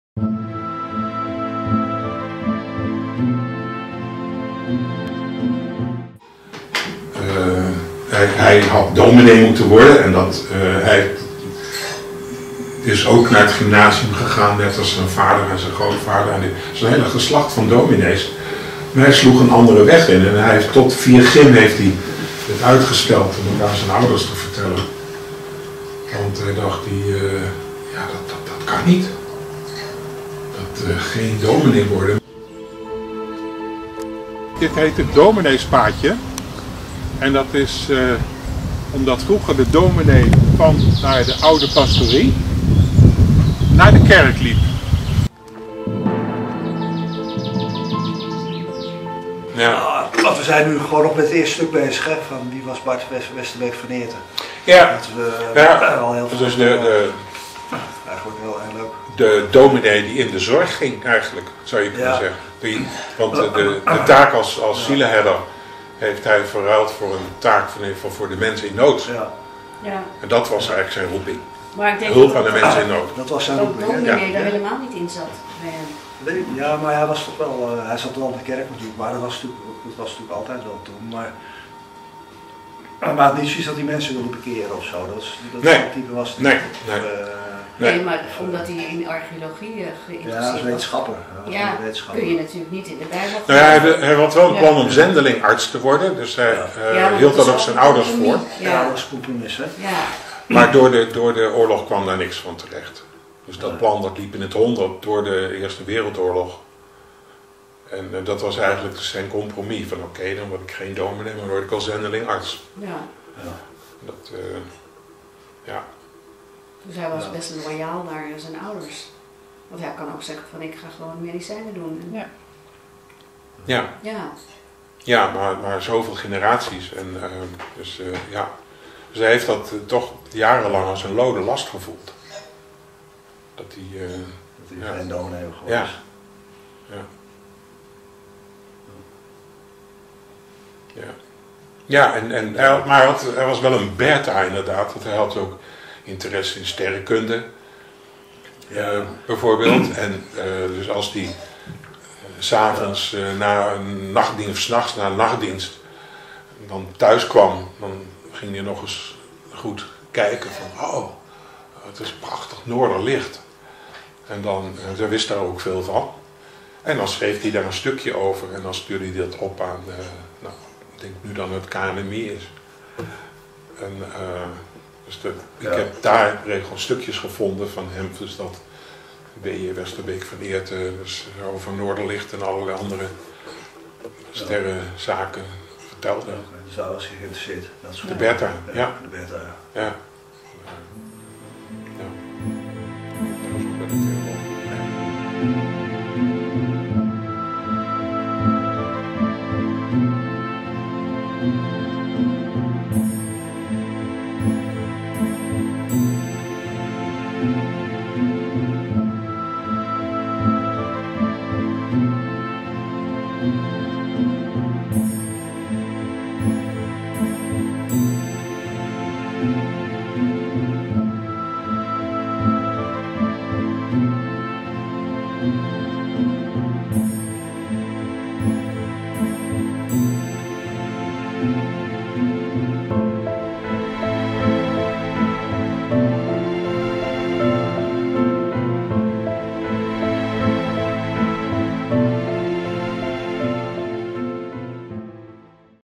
Bart Pil deel 2 jeugdjaren
Jaap van Donselaar vertelt over de jeugdjaren van zijn grootvader Bart Westerbeek van Eerten.